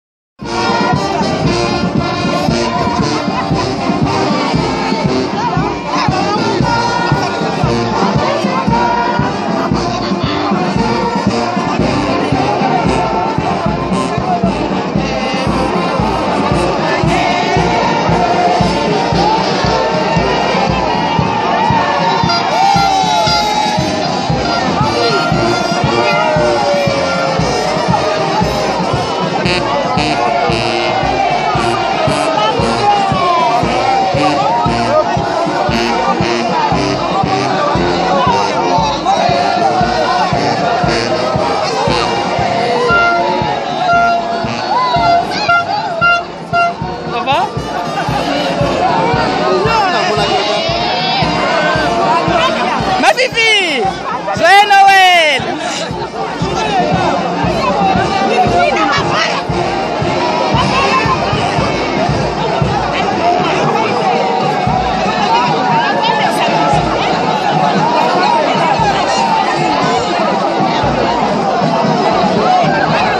Extrait ambiance du 25 mai 2015 à Paris
A l'occasion des festivités du 25 mai 2015 marquant le 99ime anniversaire de la Naissance de Papa DIALUNGANA KIANGANI, les fideles Kimbanguistes de France, se sont retrouvés pour une marche